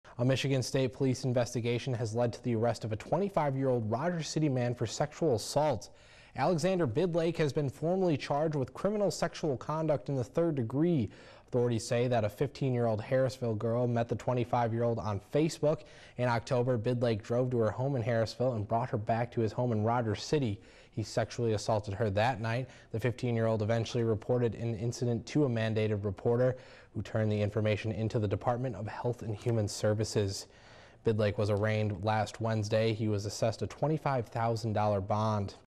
Rogers City man charged with sexually assaulting a 15-year-old girl
Rogers City, Mich. --- A Michigan State Police investigation has led to the arrest a 25–year–old Rogers City man for sexual assault.Alexander Bidlake has been formally charged with criminal sexual conduct in the 3rd degree. Authorities say that a 15–year–old Harrisville girl met the 25–year–old on Facebook.In October, Bidlake drove to her home in Harrisville and brought her back to his home in Rogers City. He sexually assaulted her that night. The victim was reported missing by her parents. She eventually reported incident to a mandated reporter who turned the information into the Department of Health and Human Services.Bidlake was arraigned in the 89th District Court last Wednesday. He was assessed a $25,000 bond.